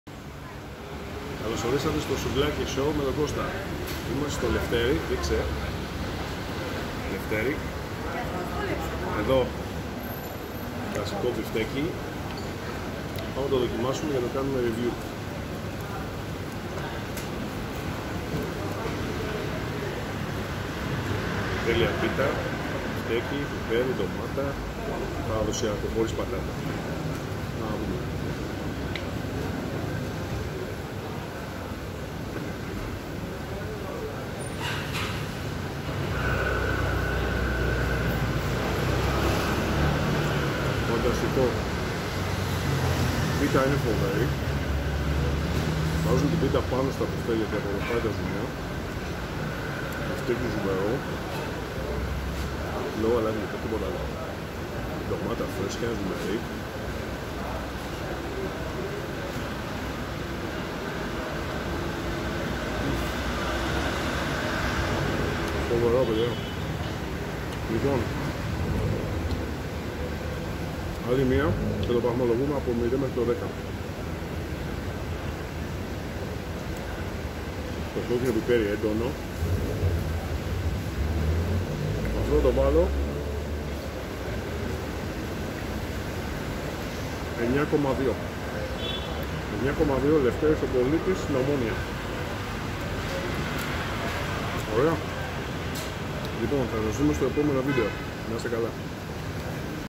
Μπιφτεκι στον Λευτερη τον πολιτη Ομονοια!!
Ενα απο τα καλυτερα μπιφτεκια στην Αθηνα.
Παραδοσιακο χωρις πατατες, ζουμερο μπιφτεκι, φρεσκια ζουμερη ντοματα,κρεμυδι,εντονο κοκκινο πιπερι, τελεια πιτα.
1.90 το τυλιχτο, το μαζαζι ειναι στην Ομονοια στην Σατοβριανδου.
Το σκορ ειναι 9.2👌👌👌🇬🇷💙